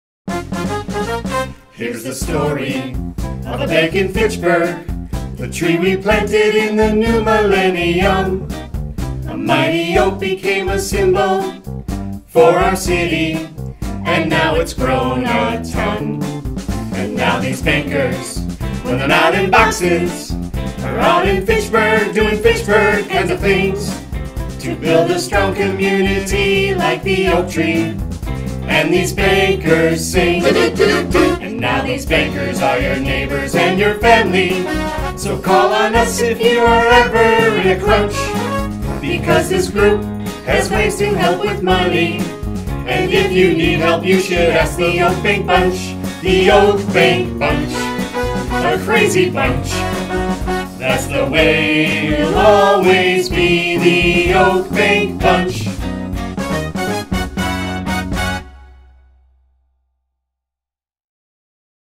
The Oak Bank Bunch!
The Oak Bank Bunch had a little fun with this Brady Bunch parody!